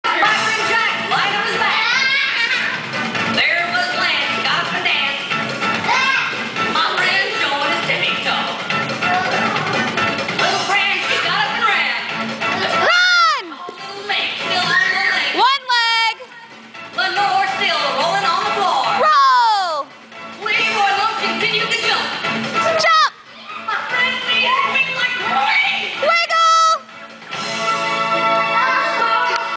Kids Birthday Party Place at The Little Gym of Friendswood TX
For more information on our birthday party activities please visit us and schedule your kids awesome birthday party!

There's no better place to celbrate a birthday than at The Little Gym of Friendswood. That's because it's incredibly fun for kids and incredibly easy for you.